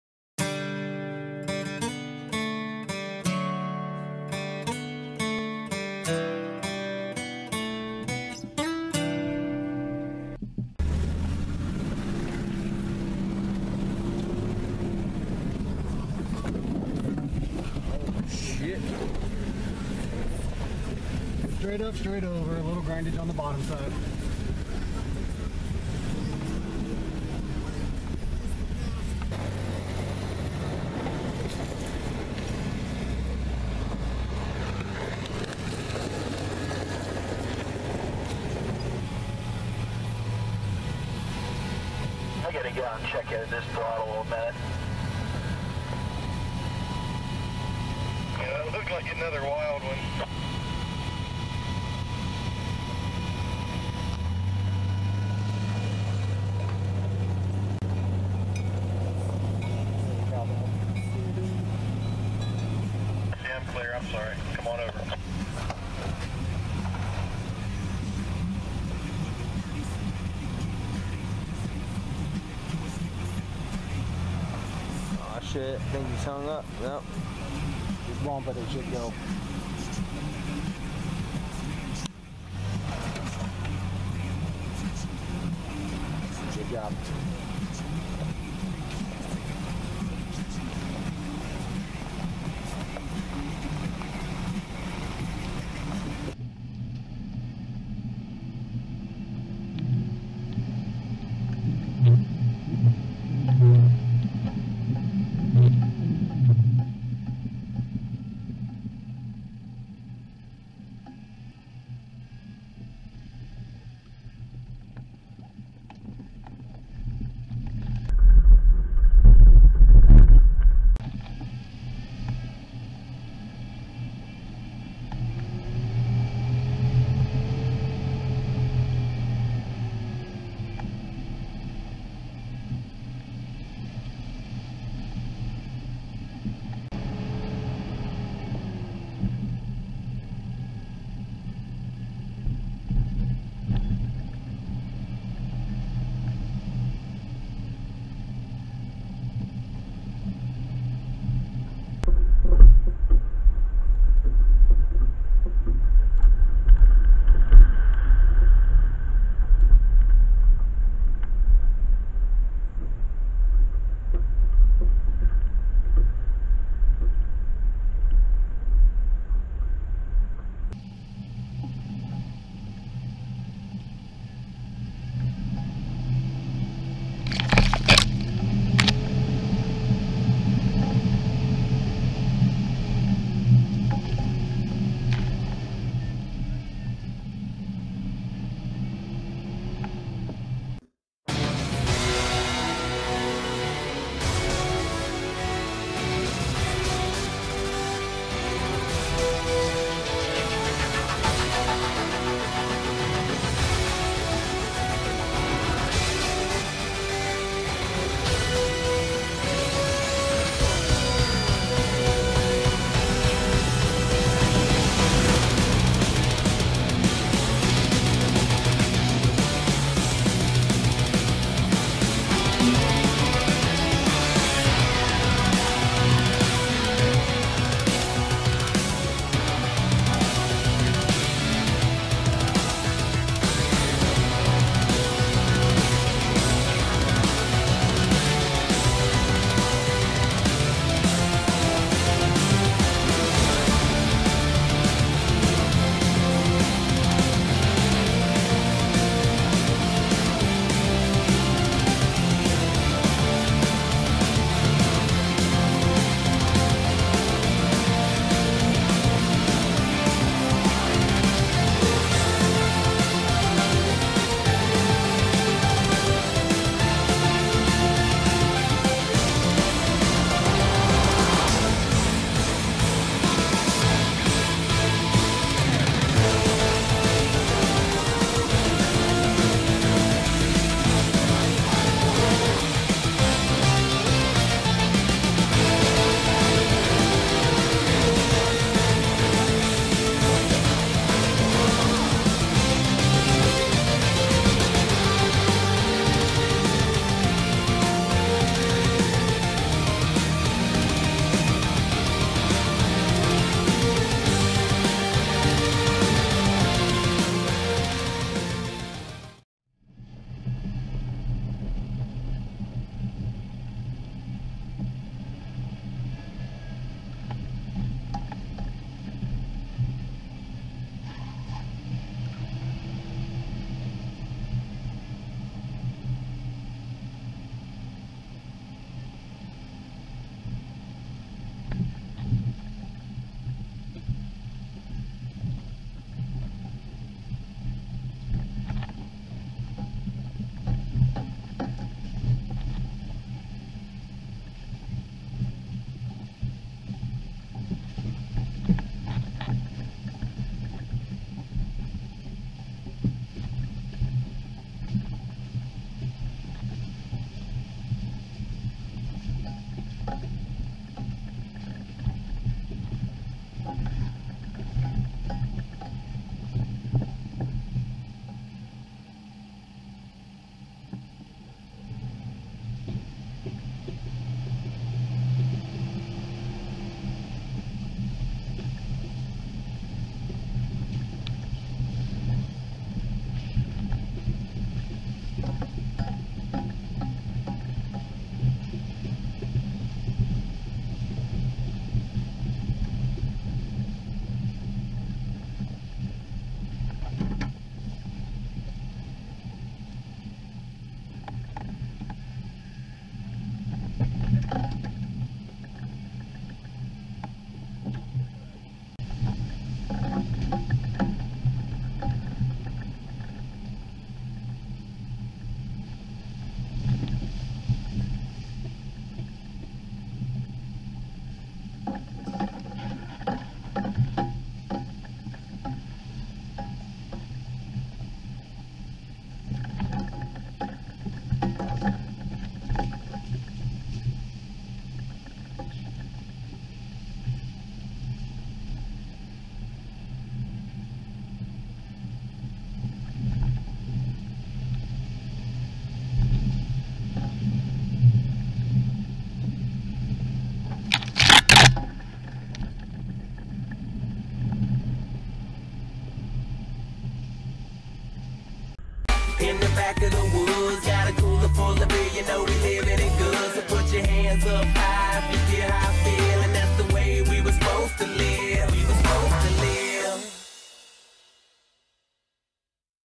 2016 Fire 'n Ice Badlands 4x4 Part 3
A little hill, a little dunes, and a little creek cruizin' at Badlands Off Road Park in Attica, IN